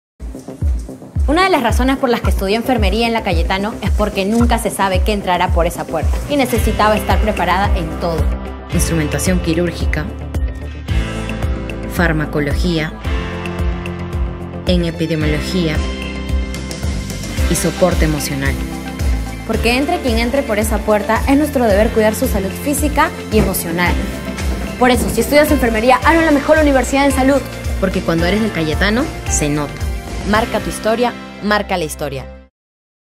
Carrera de Enfermería
Estudiando ENFERMERÍA te convertirás en un profesional en el cuidado de la salud de las personas y contribuir a la mejora de la calidad de vida.